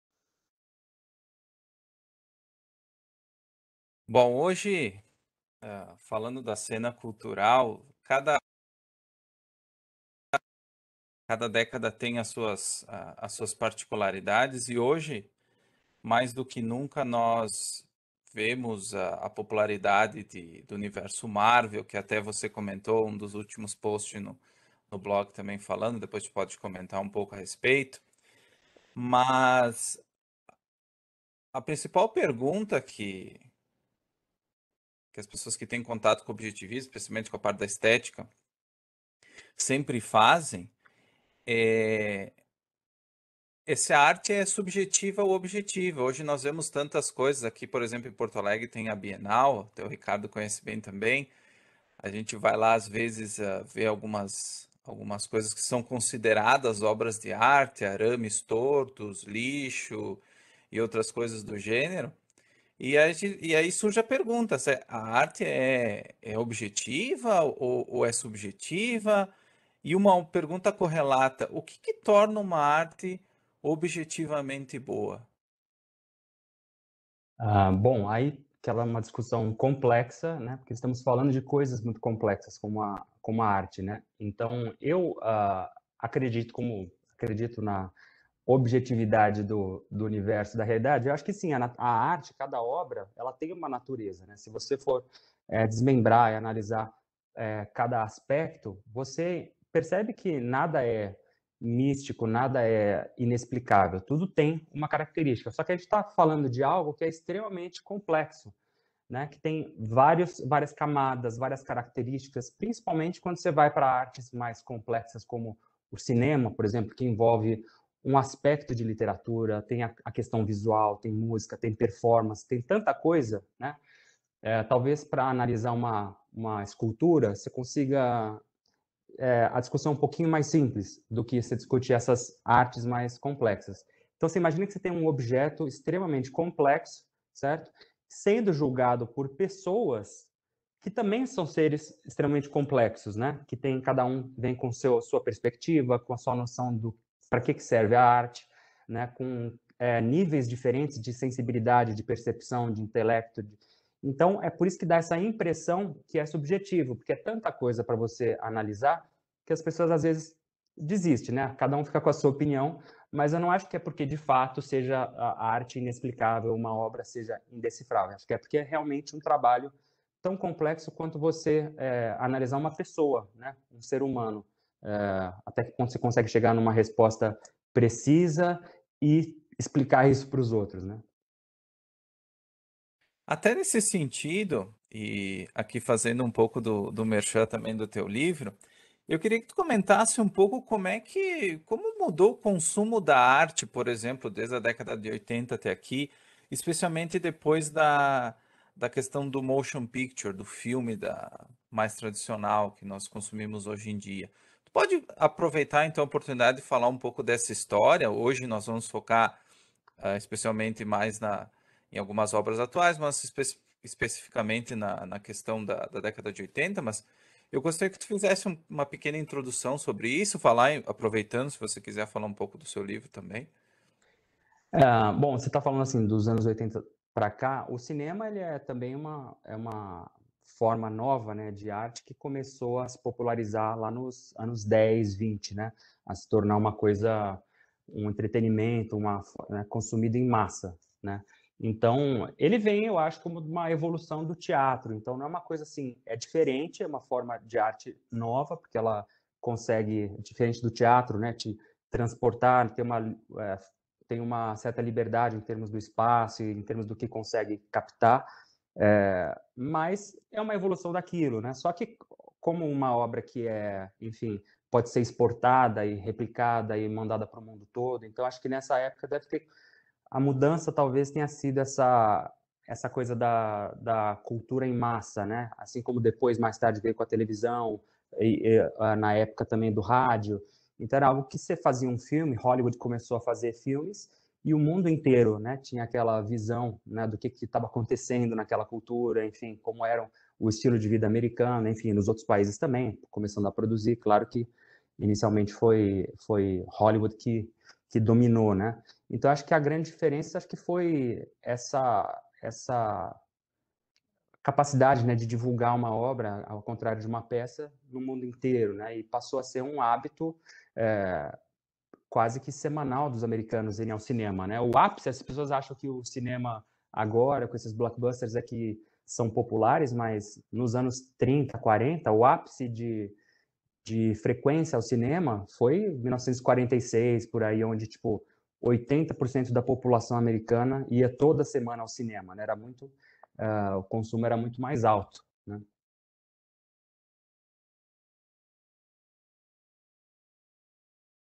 o que torna uma obra de arte objetivamente boa? | Podcast | Objetivismo Brasil
Matheus Pacini e Ricardo Birck entrevistaram Caio Amaral que comentou o que de fato faz uma arte ser algo que as pessoas gostem.